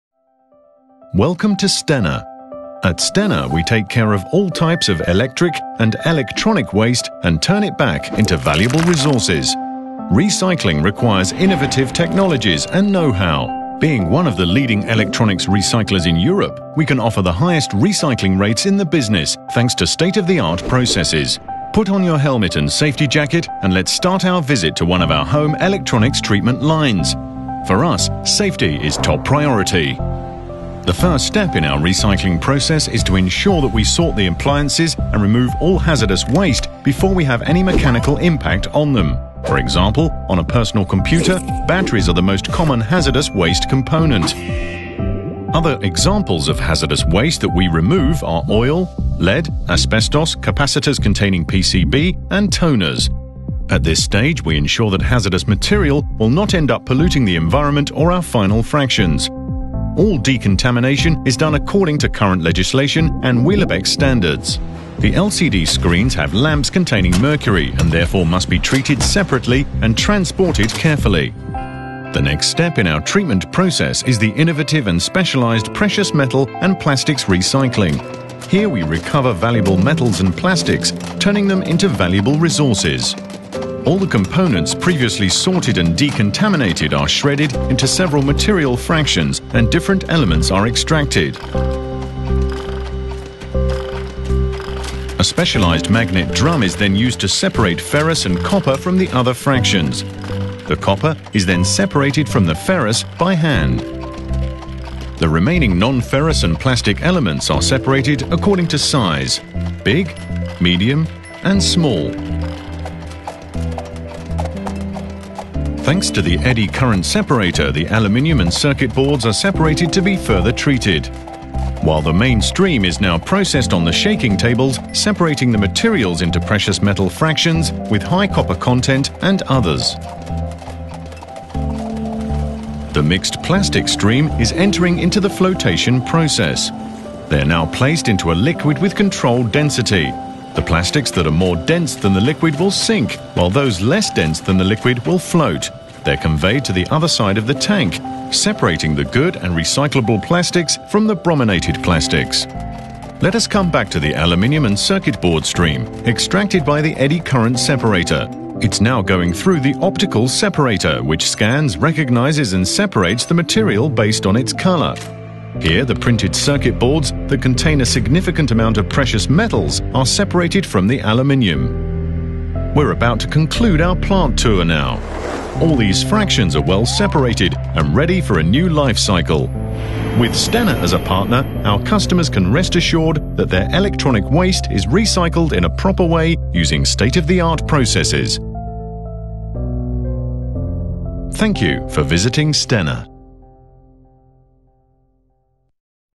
Stena Technoworld -- Electronics recycling plant
Welcome to Stena Technoworld. In our recycling plants we take care of all types of electric and electronic waste (WEEE) and turn it into new raw material. Recycling requires technology and know-how. Being one of the leading electronics recyclers in Europe we can offer the highest recycling rates in the business thanks to our state-of-the-art processes.
Put your helmet and safety jacket on and let's start the visit of one our treatment lines for small, domestic appliances.